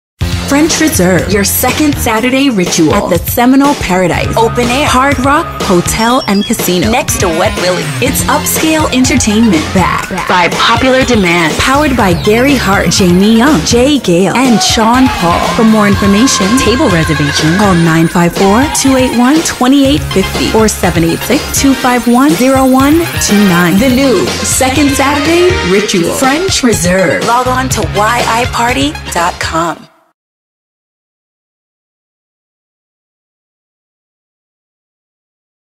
French Reserve Party at the Hardrock every Second Saturday
The new you participating in the art of partying at

FRENCH RESERVE

featuring: Richie D, Supa Dups, & SelectaAJ

21&older

New Location Seminole Paradise (Open Air + Hard Rock Hollywood)

SEMINOLE OKALEE INDIAN VILLAGE (HARDROCK) (Next to Wet Willies) 5716 Seminole Way, Hollywood FL, 33314

Celebrating Life with Jason Gayle , Selecta AJ, DJKM, & more! Happy Birthday

Upscale Entertainment Back by Popular demand. Proper Attire is Required! Expect us to exceed your expectations again! The NEW YOU! SAVE THE DATE for the New Second Saturday Ritual! Powered by Gary Hart, Jaime Young, Jay Gayle, & Sean Paul.

MIXTAPE version 1 90 Hip Hop & R&B MIXTAPE version 2 Old School Hip Hop Mixtape